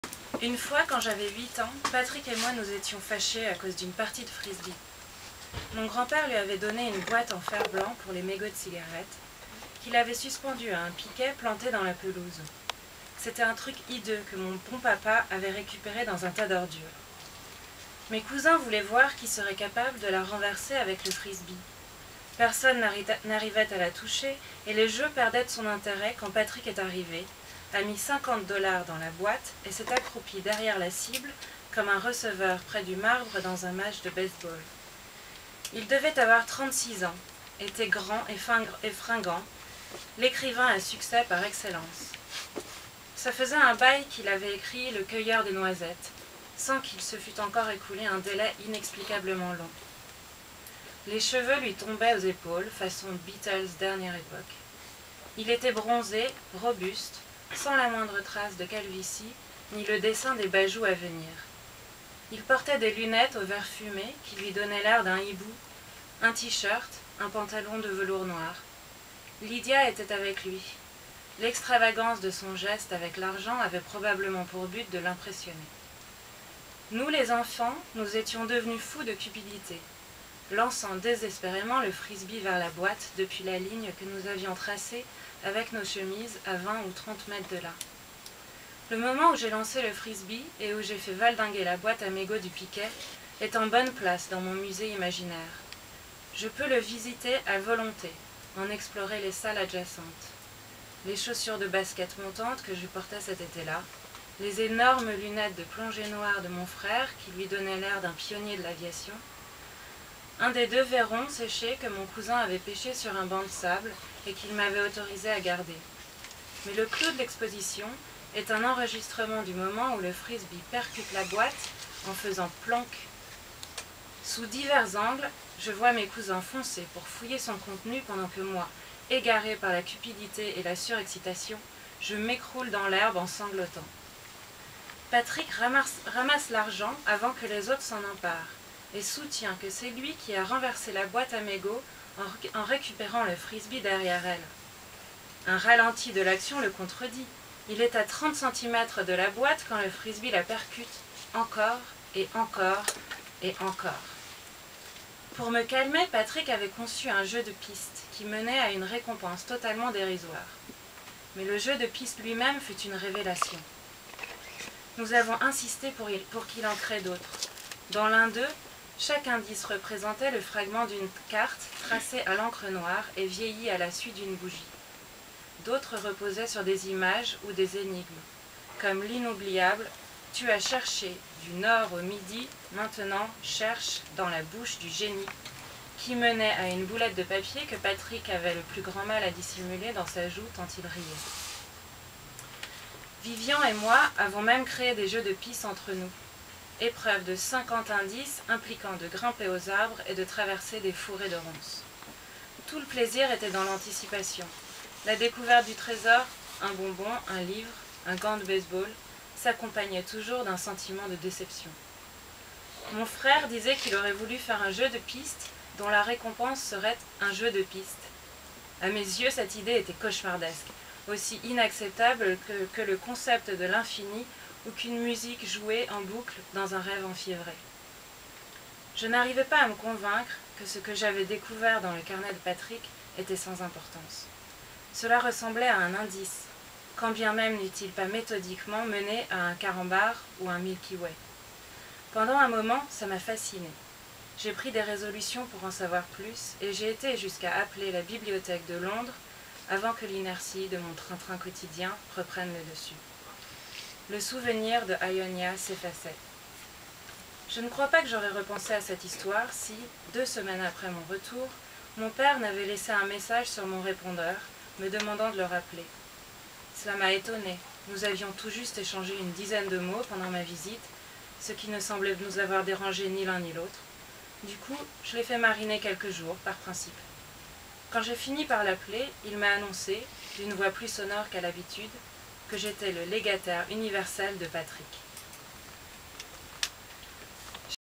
The Paperchase - French reading at Shakespeare & Company
Alice Retif reading from Marcel Theroux's "The Paperchase", recently published in French by Plon as "Jeu de Piste"